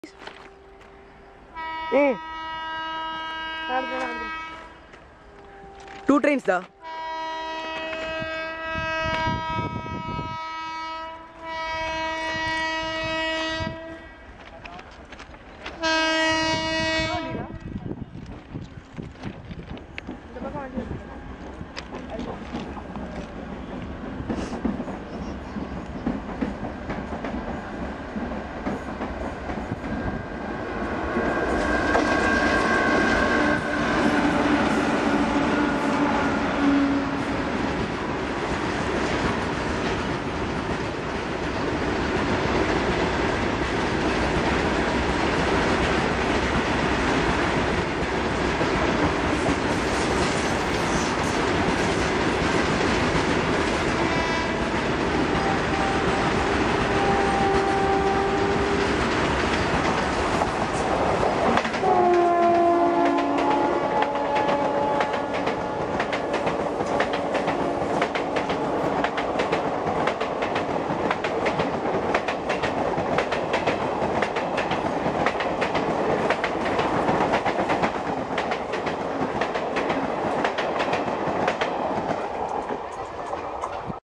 ED WAG7 Twins VS AJJ WAM4
ED WAG7 Twins BOXN crosses AJJ WAM4 Garudathri